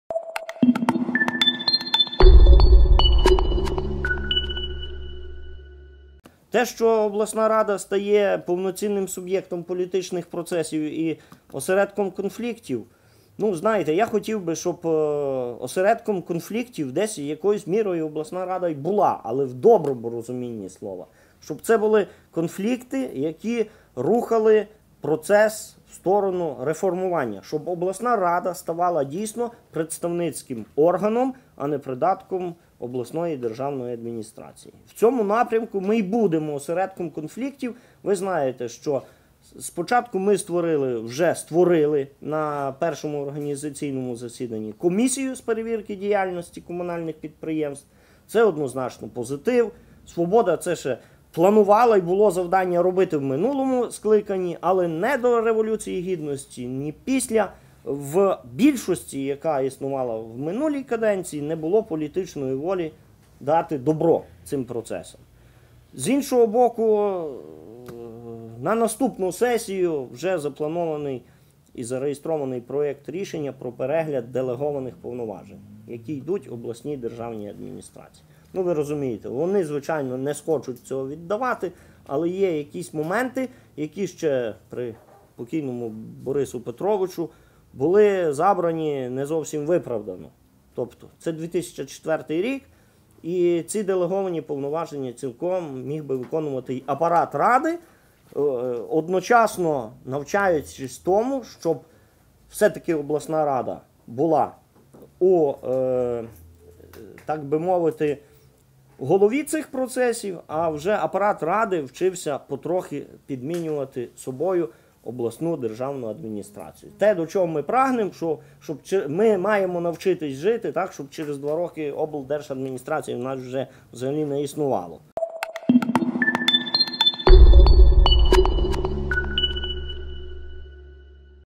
Обласна рада буде осередком конфліктів, - Олександр Пирожик
Волинська обласна рада й надалі буде осередком конфліктів, - вважає заступник її голови Олександр Пирожик. Про це він заявив на засіданні круглого столу за участю громадських експертів, коли ті підбивали політичні підсумки 2015 року на Волині.